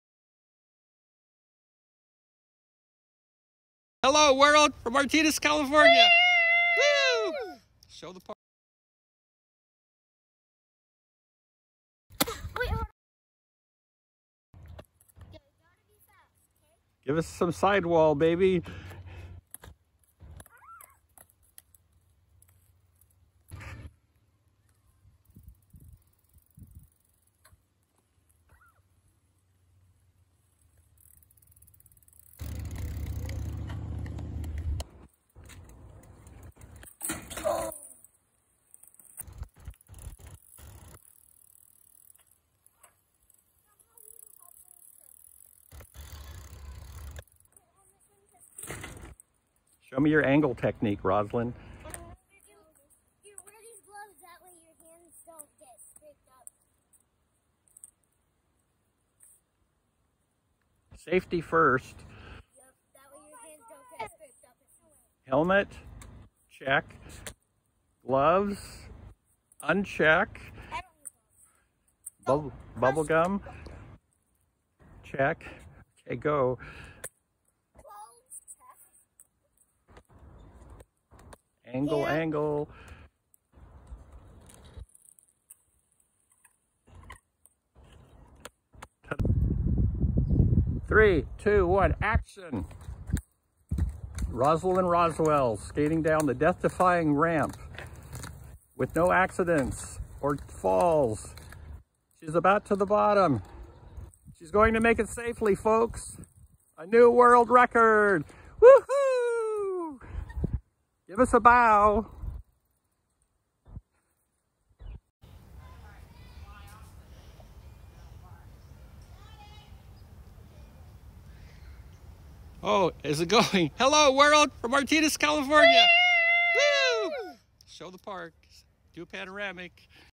Grandkids Cut It Up at Roller Park
July 27, 2024 - Grandpa Rob, Johnnie, and Rosalyn "Cutting It Up at Roller Park". Practicing their bicycling and rollerblading skills.